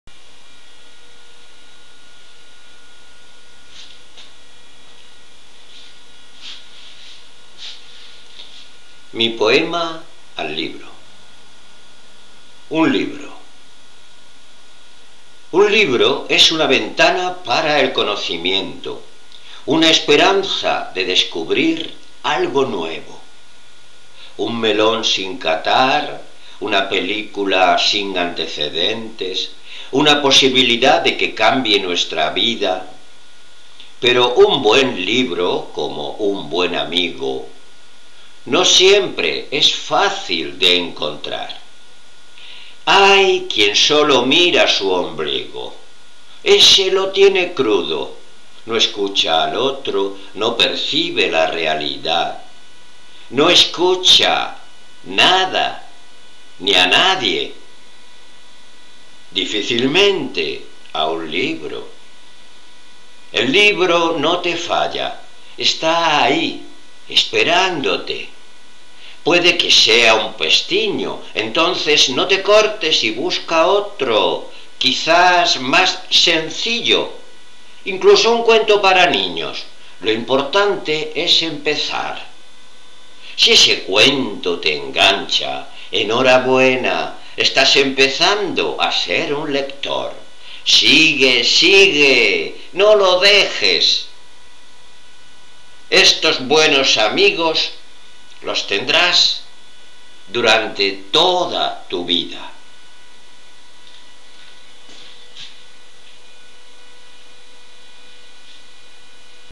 Rumboso2 . UN LIBRO. ( POEMA).
"Quería subir una frase y me ha salido un poema, lo importante para mí es buscar ocasiones para despertar en aquellos que no leen o leen sin ilusión y a veces por obligación, el placer de los LIBROS."